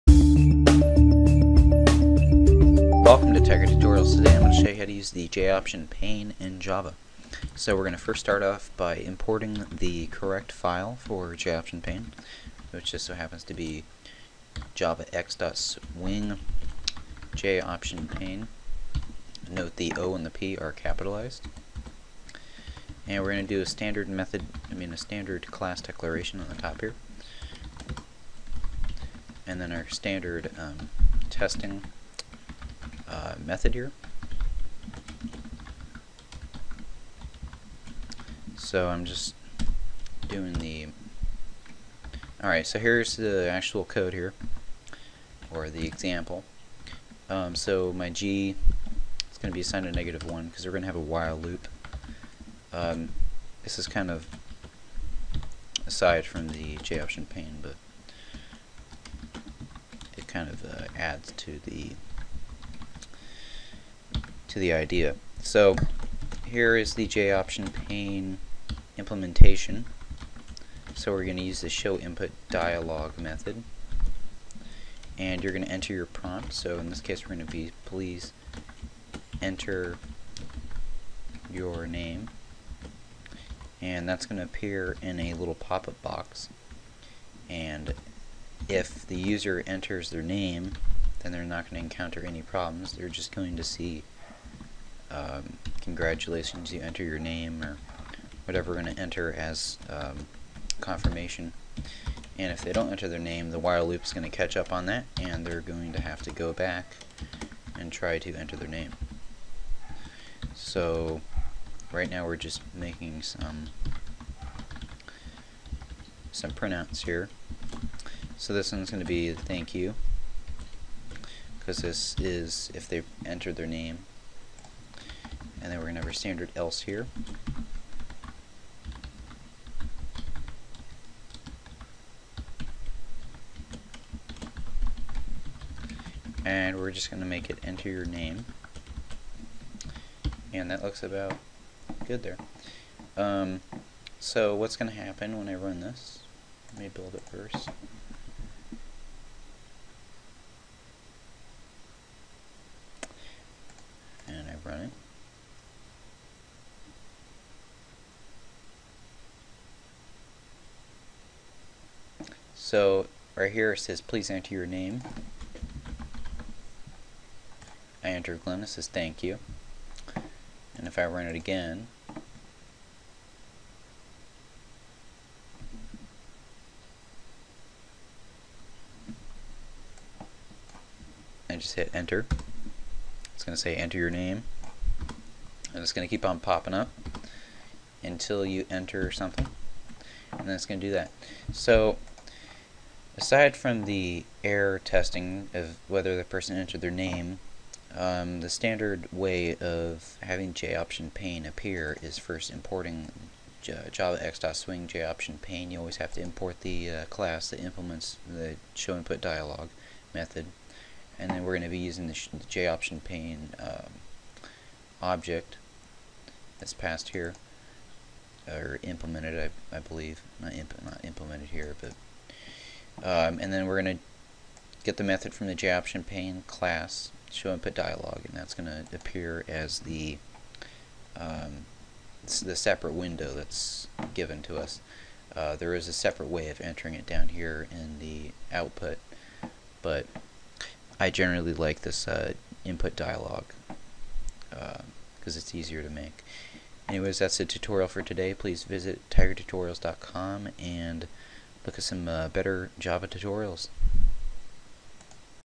JOptionPane - Java Basics
Prompt a user to enter his or her information using the JOptionPane.showInputDialog() method. Be sure to first import the class:

] for more java and website tutorials!